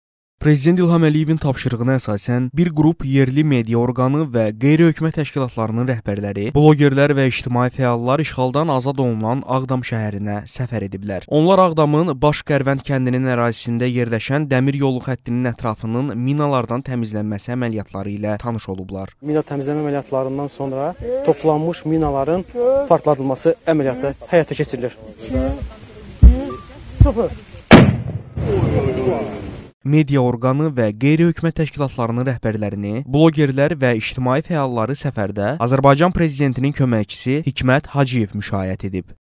Media nümayəndələri Ağdamda
Media rəhbərləri minatəmizləmə əməliyyatları ilə tanış olublar